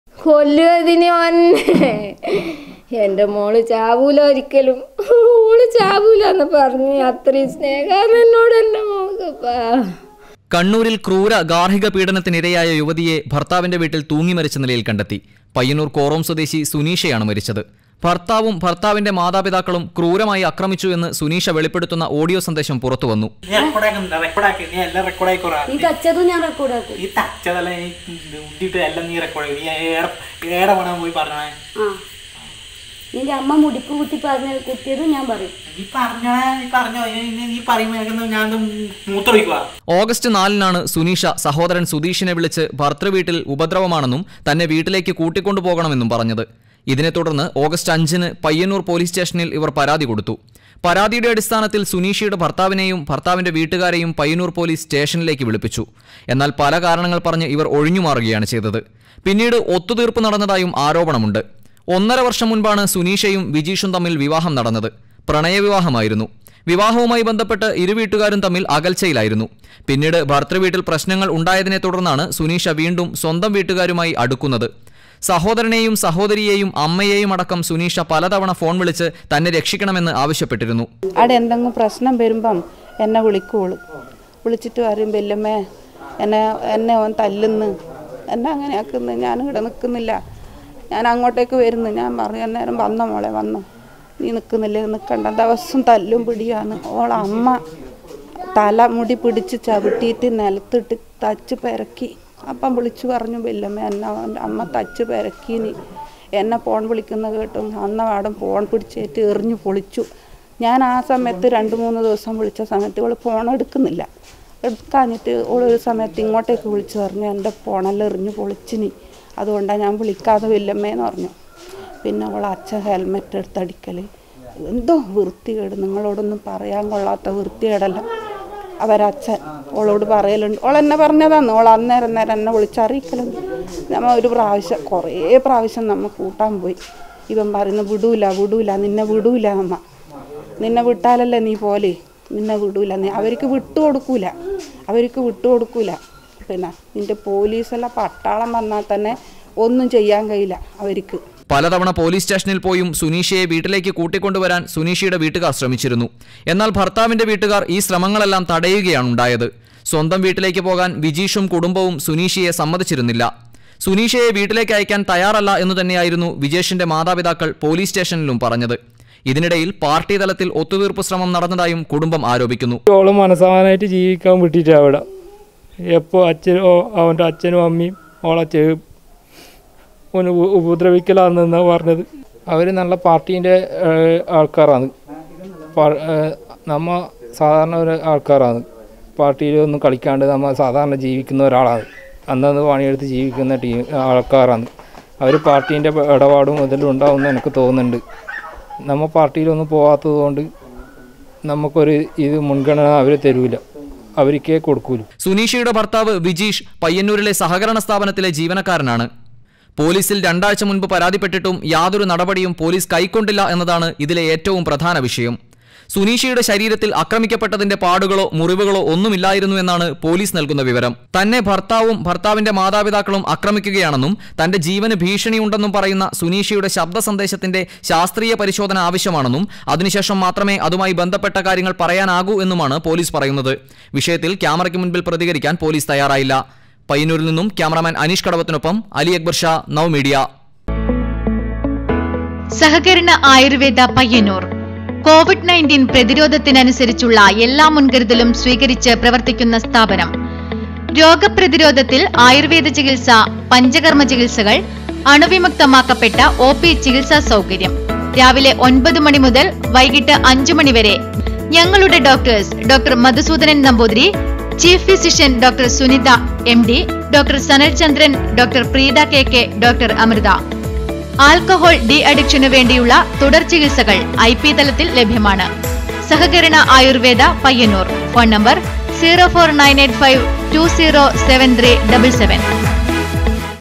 ഗാര്‍ഹിക പീഡനം, പയ്യന്നൂരിലെ സുനീഷയുടെ മരണം കൊലപാതകമെന്ന് കുടുംബം
ഗാര്‍ഹിക പീഡനം, പയ്യന്നൂരിലെ സുനീഷയുടെ മരണം കൊലപാതകമെന്ന് കുടുംബം. ഭര്‍ത്താവും ഭര്‍ത്താവിന്റെ മാതാപിതാക്കളും ക്രൂരമായി തല്ലിച്ചതച്ചു എന്ന് സുനീഷ വെളിപ്പെടുത്തുന്ന ശബ്ദ സന്ദേശം പുറത്ത്